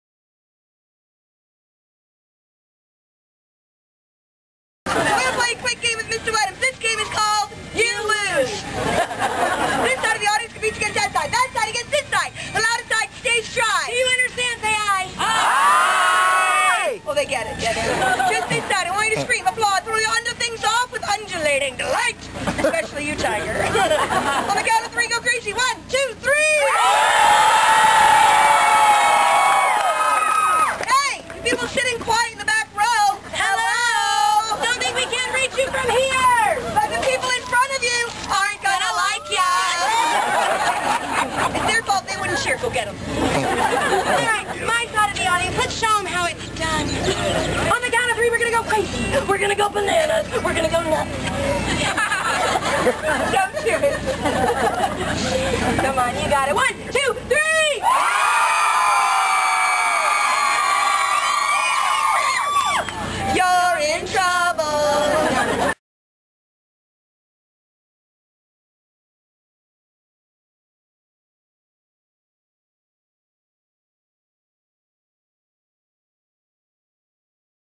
Washing Well Wenches at the 2016 Renaissance Pleasure Faire
I had not been to the Renaissance Faire in five years. This was a new act for me, and pretty funny it was. Unfortunately, I only got this portion and a brief snippet of another part of the show. It becomes a toss-up as to whether to shoot the entire show -- which can go on for twenty minutes or more -- or get portions of it and hope one got a good sample of what their act was like. I opted for the latter. I've shot entire shows in the past with my VHS camcorder, so this is a better choice in the long run. Check out my photos on Facebook and Flickr of this and the rest of the Faire.